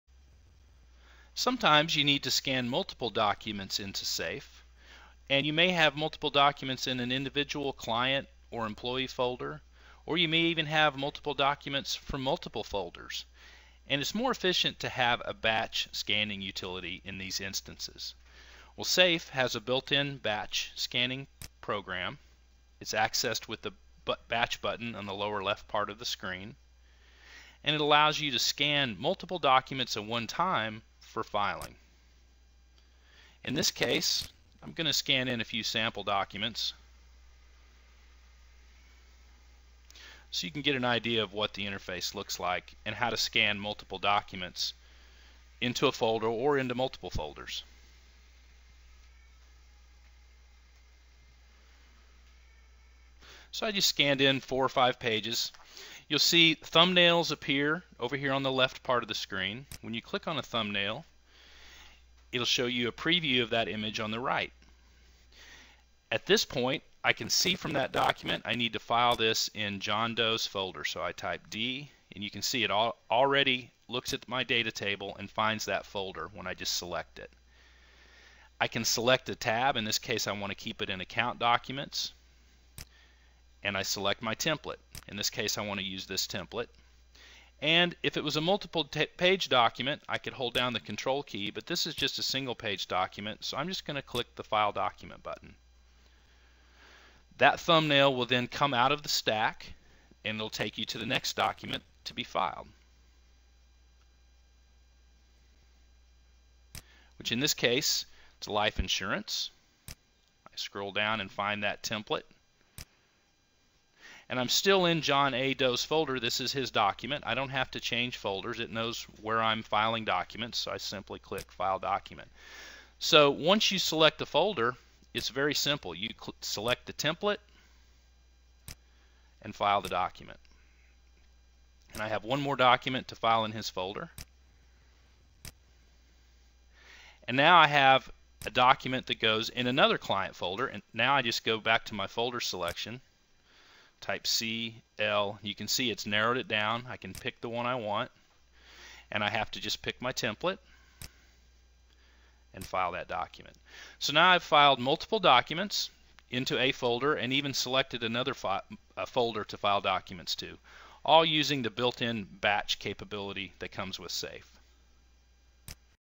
Cabinet Demo: Point-and-Click Batch Scanning & Indexing
When you need to scan and file multiple documents in a batch process, the SAFE Batch module offers efficient point and click operation.